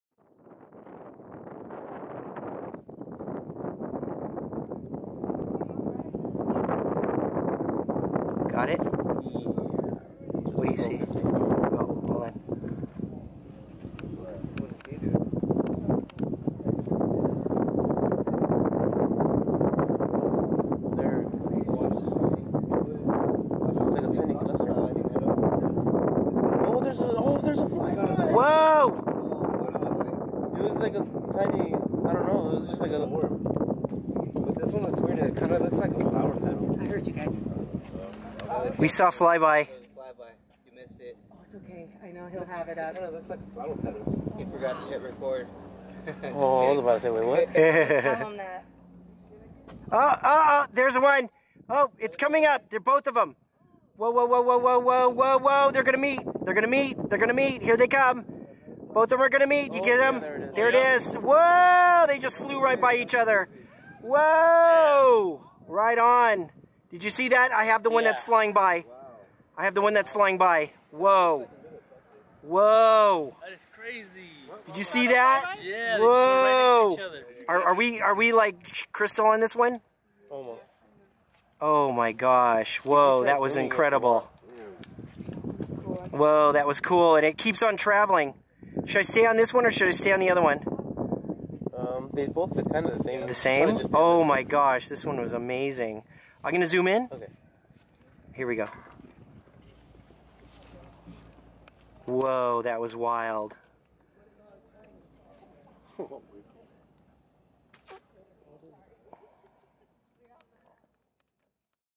UFO sighting event - A stationary with two flybys 7/23/17
UFO Sighting event yields over 30 objects this day.  Multiple eyewitnesses saw a stationary object get two flybys.  Many other fantastic phenomena happened this day.  It was awesome to show this experience with you guys.